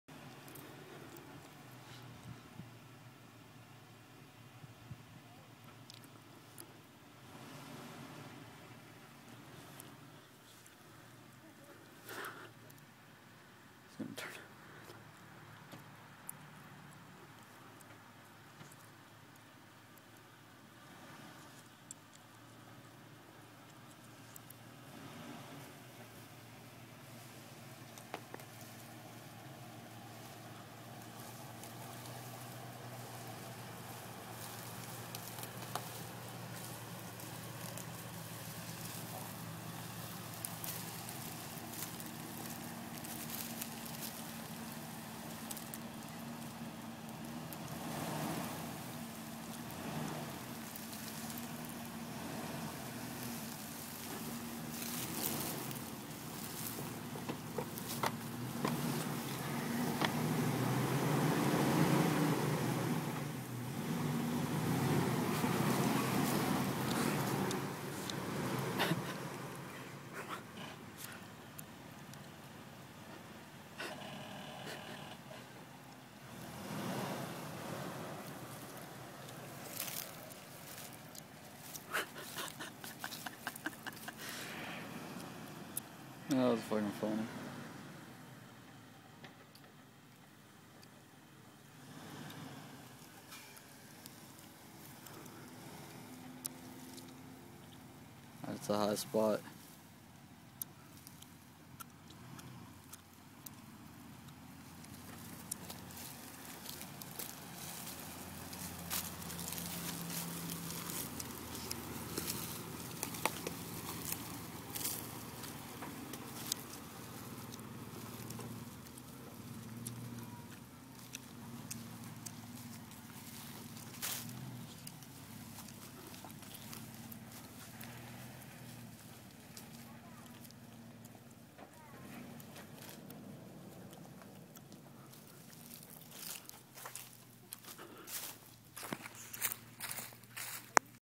Why I want lockers
Off roading a little on Andrew's farm.